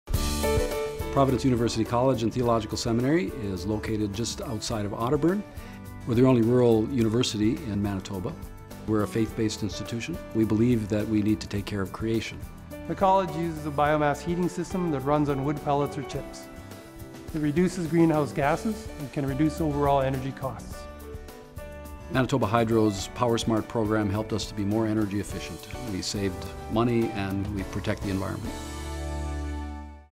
Manitoba Hydro Commercial - Biomass Heating System
This commercial was produced by Manitoba Hydro to explain our biomass heating system, and how it helps the environment.